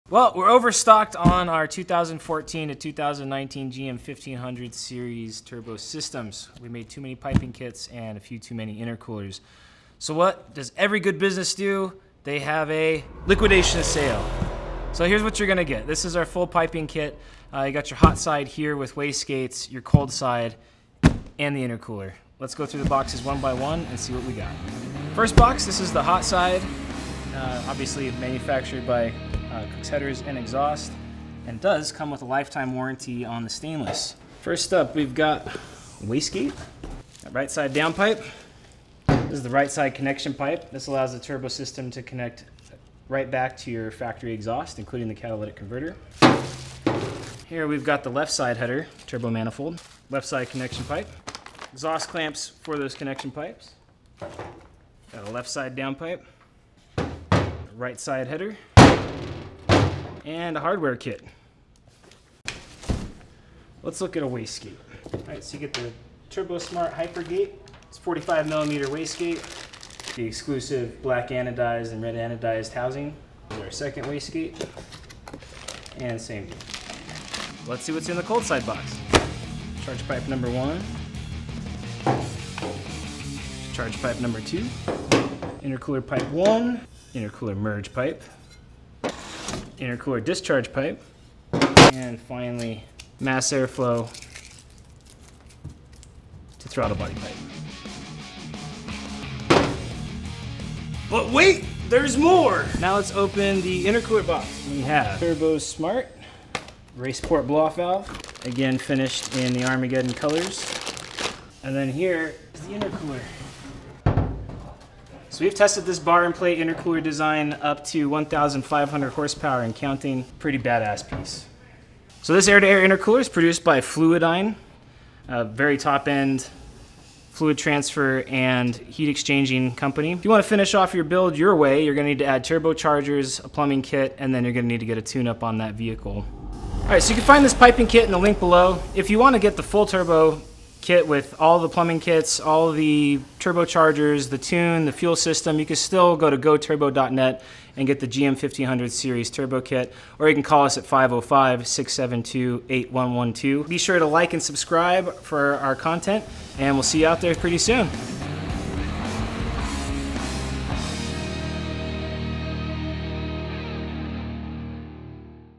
Unboxing The 1500+ HP GM Turbo Kit
This is the Armageddon GM Turbo Kit unboxing! We had a ton of fun with this thing and it's only just begun. We made too many, so now you can get the turbo piping that put out over 1500 horsepower on 5.3L and EcoTec3 6.2L GM Engines.

HOTSIDE KIT:
-304 Stainless Steel Headers: 14 Gauge Primaries, 16 Gauge Collectors, 14 Gauge Up-Pipe
-304 Stainless Steel T4 Flange
-304 Stainless Steel Down-Pipe
-Vibrant Bellows on Wastegate Recirc: Part # 68425
-Header Bolt Kit
-MLS Header Gaskets
-x8 M10 X 1.5 X 38mm FLANGE BOLT - 8PCS
-x8 M10 X 1.5 FLANGE NUT - 8PCS
-x3 M10 X 1.5 X 38mm FLANGE BOLT - 3PCS
-x3 M10 X 1.5 X 50mm FLANGE BOLT - 3PCS
-x3 M10 X 1.5 FLANGE NUT - 6PCS
-x2 Wastegate Weld Flanges (Provided by Armageddon)
-x2 Armageddon Serial-Numbered Badges on Downpipes (Serial Number Starting 20-1001)

COLD SIDE PIPING

-2x 2.5” Aluminum Turbo to Merge Tubes
-1x 3” by (2x) 2.5” Aluminum Merge Tube
-1x 3.5” Intercooler to MAF Housing Tube
-1x 3.5” MAF Housing to Throttle Body Tube w/ MAF Block
-2x 8-32 x 12mm Bolts w/ Lock Washers for MAF Block
-2x 8 x 1.25 x 12mm Flange Bolt
-2x 8 x 1.25mm Flange Nut
-1x Aluminum Armageddon Badge Welded to MAF Tube (Provided By Armageddon)